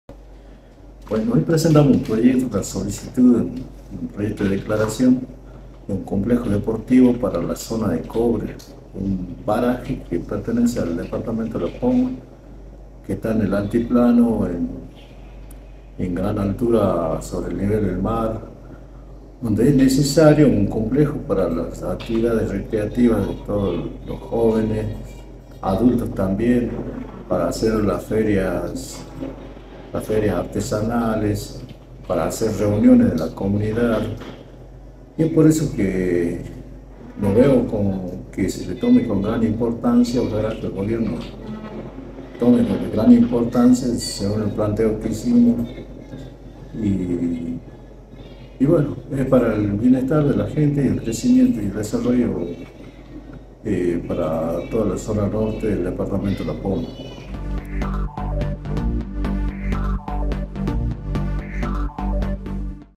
SALTA - Solicitan la construcción de un complejo deportivo en la localidad de Cobres
Ernesto Tapia - Diputado de La Poma